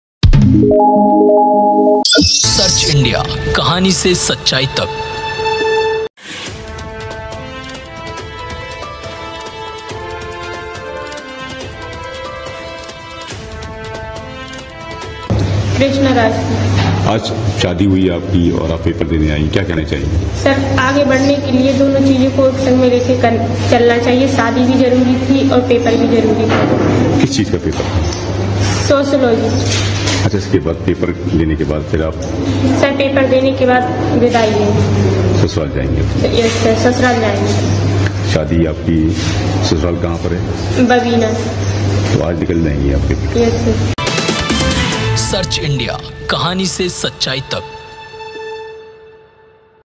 सात फेरों के बाद परीक्षा देने पहुंची दुल्हन..!
दुल्हन कृष्णा राजपूत ने बताया कि आगे बढ़ने के लिए दोनों चीजे साथ में लेकर चलना चाहिए. शादी भी जरूरी थी और पेपर भी जरूरी थे. पेपर देने के बाद विदाई l